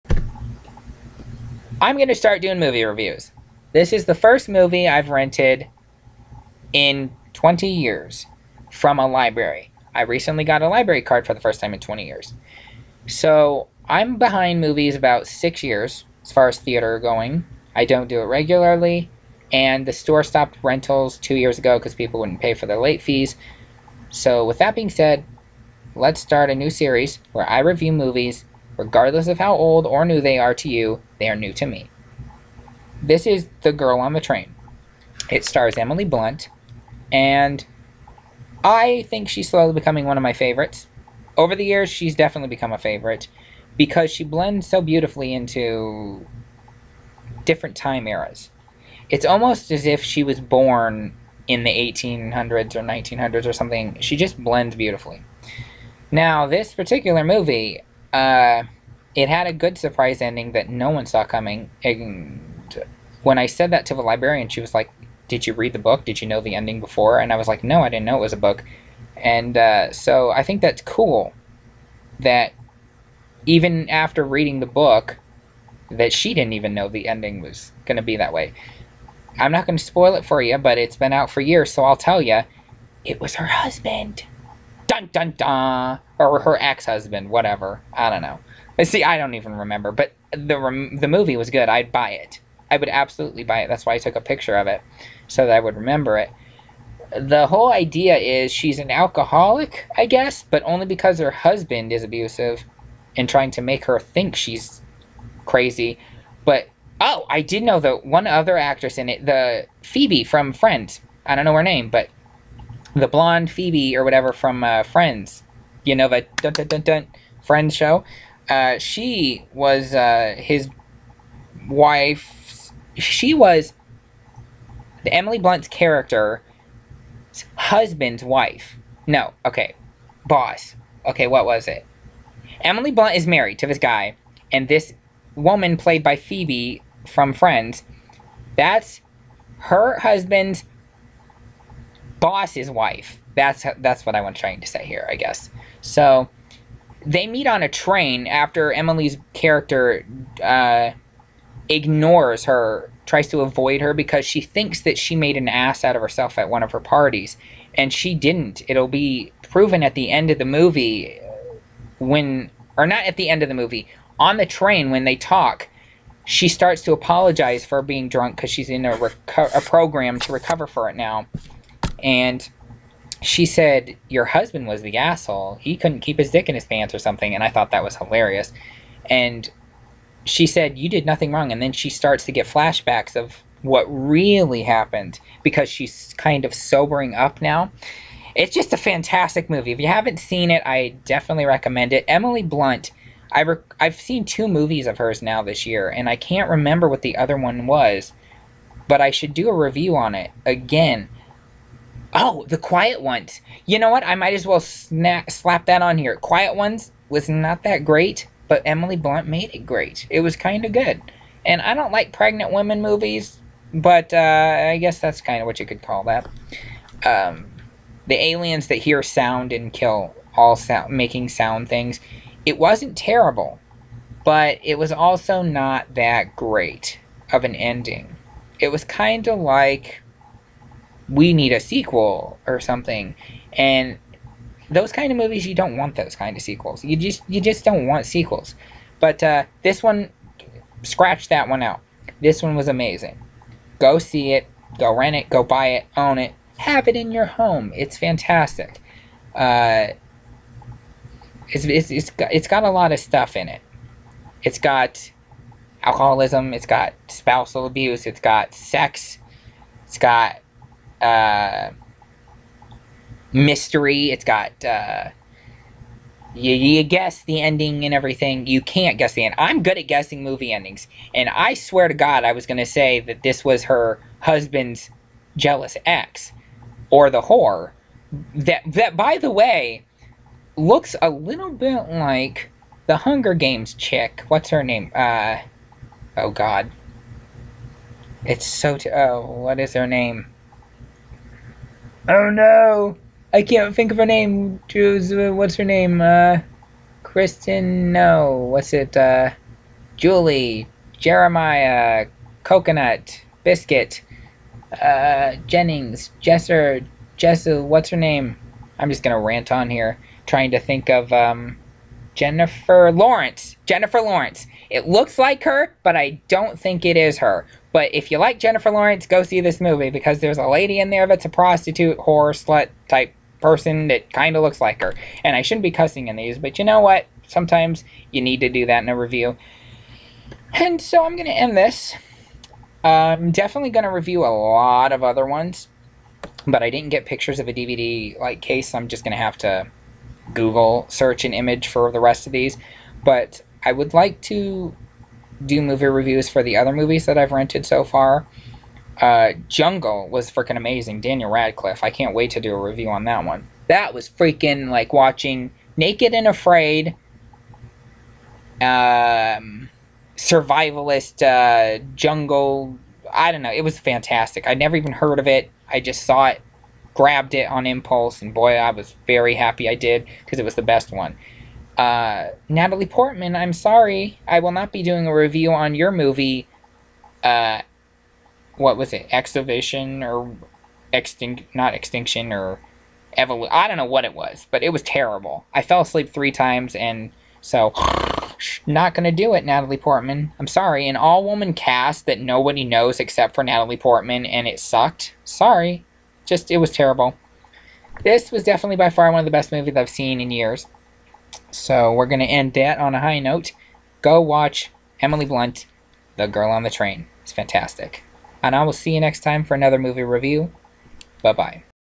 The Girl On The Train Movie Review
This is a new series I will be doing as I have explained. I have watched over 100 movies since April 2019 and I will be doing 96 or so reviews in this series.  I have already recorded 9 videos and 8 are different movies and 1 is a combo of several movies I didn't find interesting enough to do full reviews of. I hope these will get better and trust me I have a lot to film so I'm sure they will get better and more relaxed.